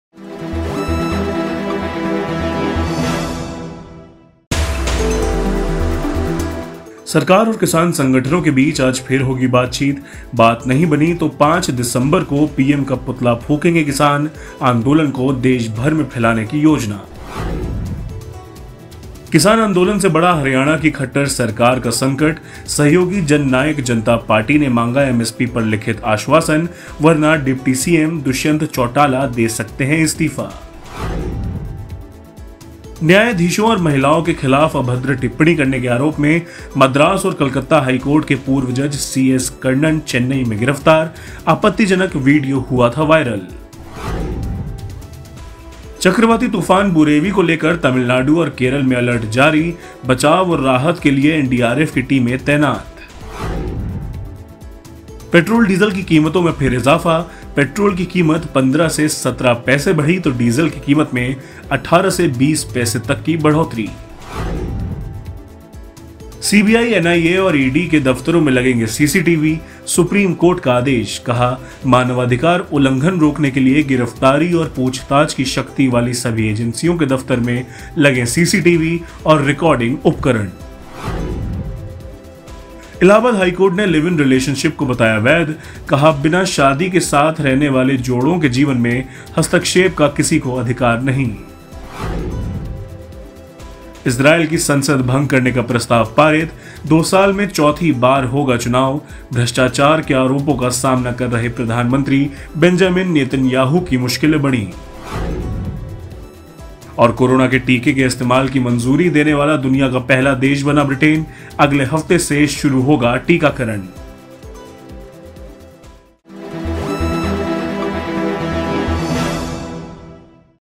Go Headlines: Top News Of The Hour
Round 4 talks today: Farmers to burn PM's effigy, intensity movement if no breakthrough; Haryana BJP ally JJP seeks written assurance on MSP, else Deputy CM Dushyant Chautala may resign; Chennai: Former HC judge CS Karnan arrested over 'offensive' remarks on judges, women; Cyclone Burevi: Red alert issued, NDRF teams deployed in Tamil Nadu, Kerala; Fuel price hike: Petrol dearer by 15-17 paise, diesel by 18-20 paise; SC directs Centre to install CCTV cameras in offices of CBI, NIA, ED – all agencies which arrest, interrogate; and more